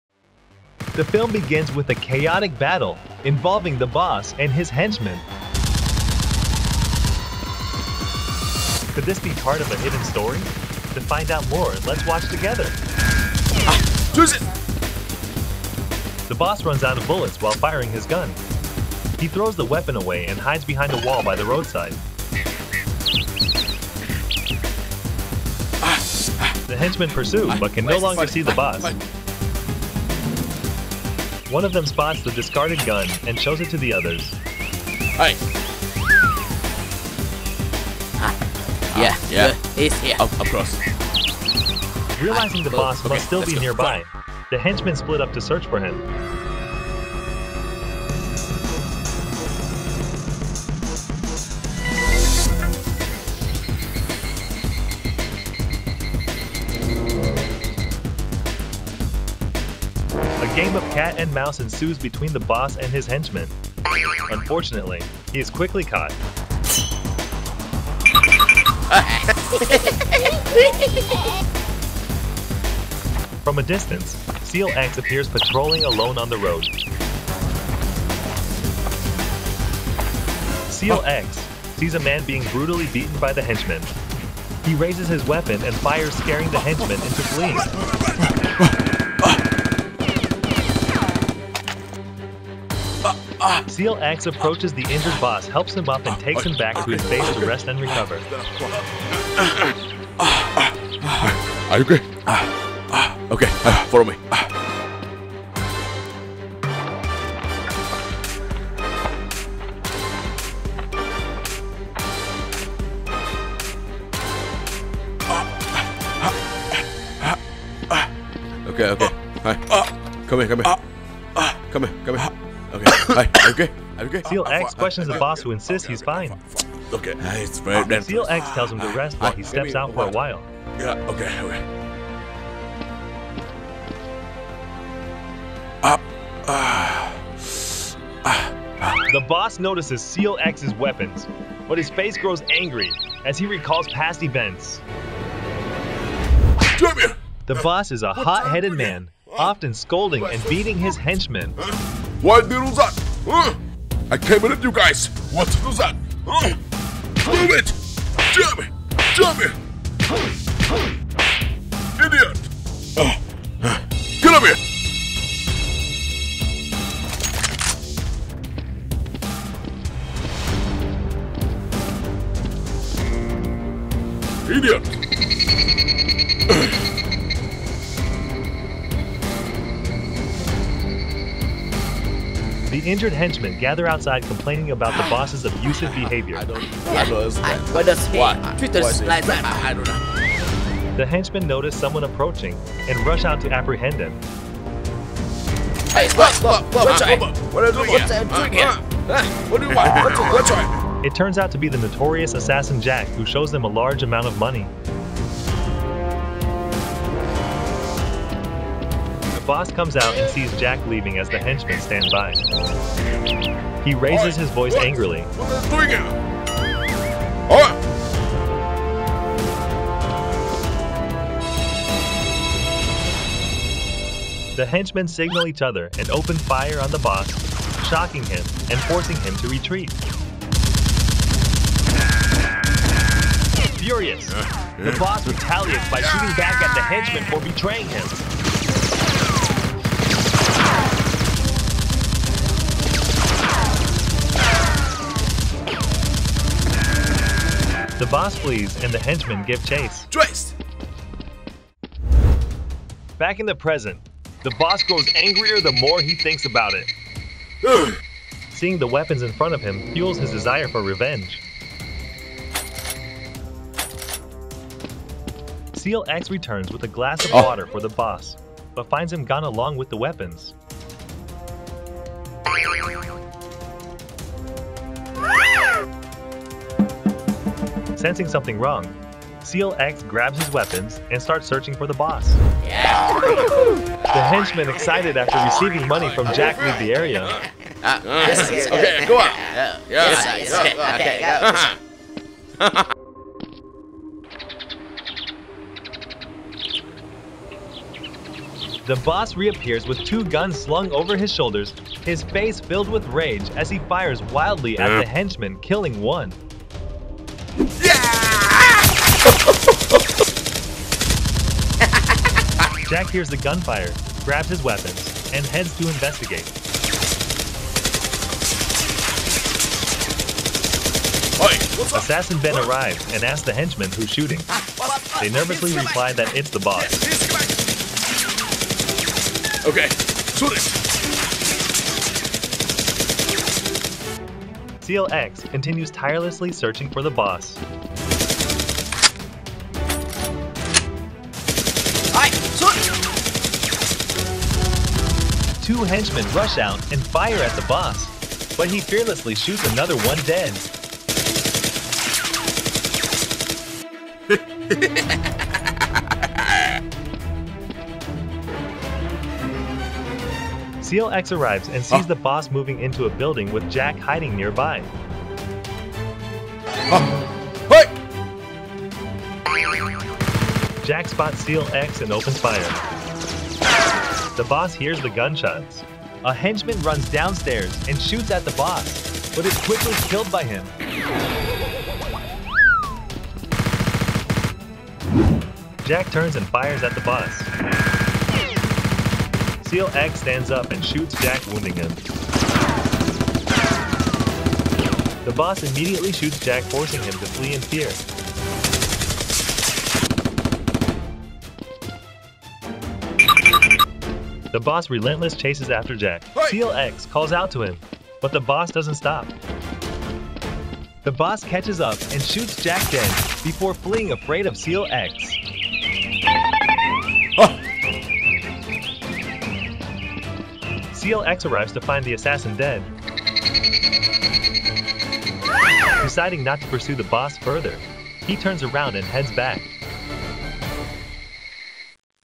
Action LTT: Special Police SEALX Warriors Nerf Guns Fight Criminal Tycoon Group SEAL X Intervenes
SEAL X confronts The Boss in this action-packed Nerf battle! Watch as revenge unfolds in this thrilling family-friendly adventure.

Experience an intense Nerf war where SEAL X discovers a man being attacked by his own team. After rescuing The Boss, SEAL X offers him shelter - but The Boss has other plans. Watch as this epic story unfolds with amazing Nerf battles, tactical moves, and surprising twists!

This action-packed video features the elite SWAT SEAL X team facing off against criminal groups in an exciting showdown. See how personal vendettas and betrayal lead to an explosive confrontation between The Boss, his former team, and the mysterious assassin Jack.

Watch as SEAL X tries to maintain peace while The Boss seeks revenge in this high-stakes Nerf battle. With amazing tactical moves and intense action sequences, this family-friendly adventure will keep you on the edge of your seat!

00:00 - The Boss and His Henchmen Betrayal
01:19 - Seal X Saves the Boss from Danger
03:20 - Assassin Jack Bribes the Henchmen
04:37 - The Boss Seeks Revenge on Betrayers
05:34 - Seal X vs The Boss and Assassin Jack Showdown